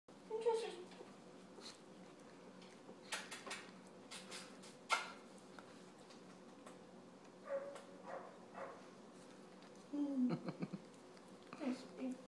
Golden Retriever puppy ~ tail wagging fun!
This is an ecstatic almost-six-week-old golden retriever puppy.